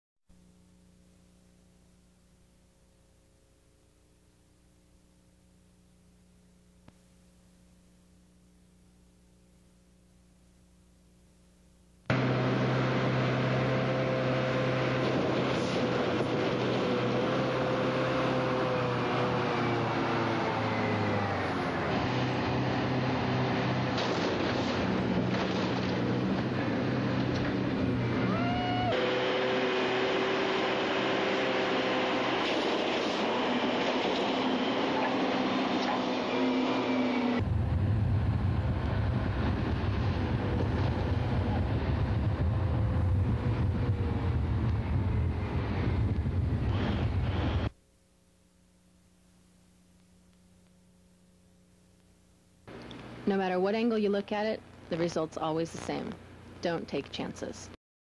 CAT Haul Truck flattens pickup truck!
A demonstration to show how easily a haul truck can drive over a light vehicle,